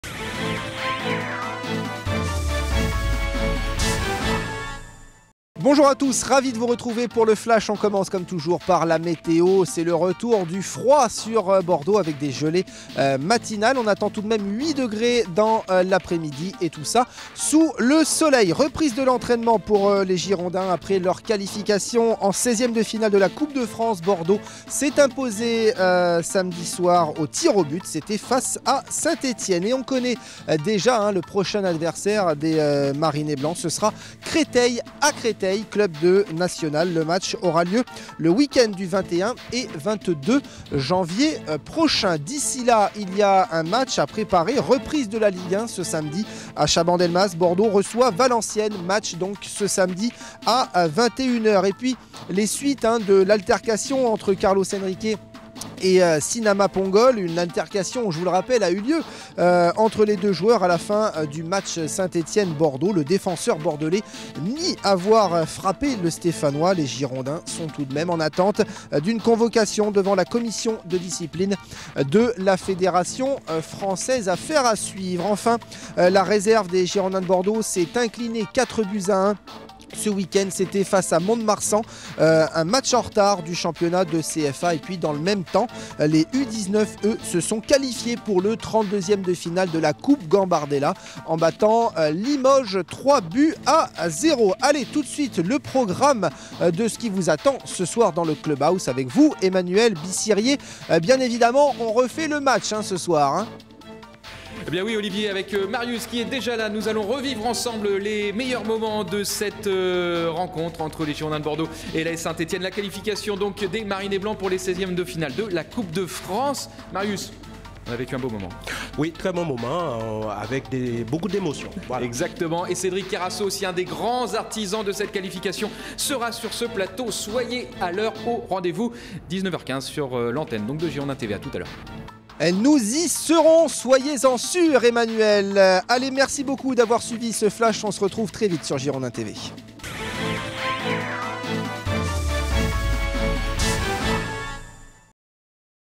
Les infos du 10 janvier 2012
Olivier Faur présente la 1ère édition de la semaine du Flash Info. Il évoque l'actualité du week-end des Girondins de Bordeaux et notamment la qualification pour les 16èmes de finale de Coupe de France.